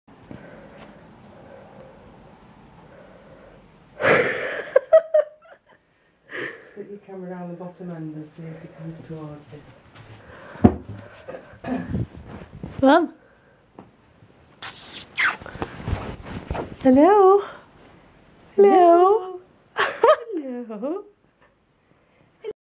Truffle inside the duvet cover
Boycat decides to help us make the bed!